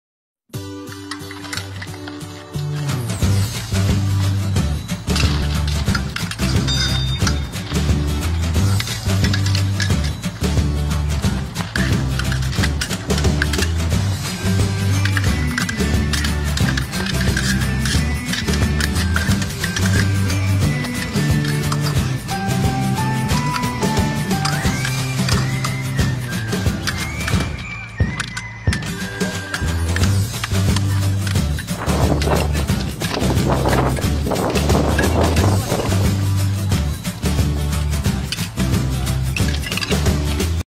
Открыли сезон занятий на свежем воздухе 2019
Вчера открыли сезон занятий на свежем воздухе - погода позволяет!😎👊👍. График тренировок может меняться - внимательно смотрите инф в группе ФБГ в WhatsApp.